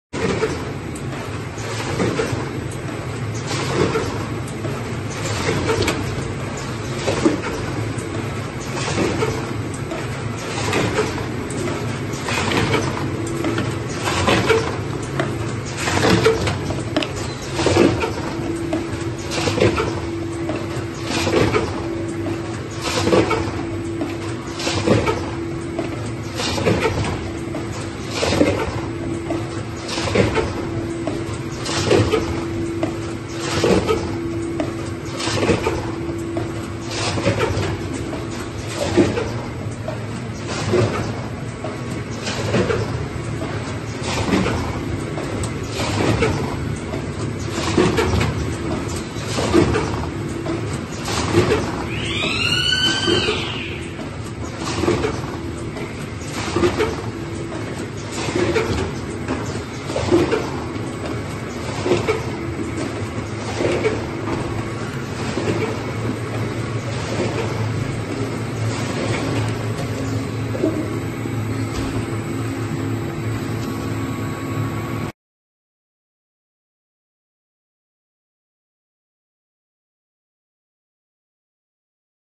ENVASEF: integración de pesadora multicabezal con llenadora de botes
Integración de pesadora multicabezal con llenadora de botes de snacks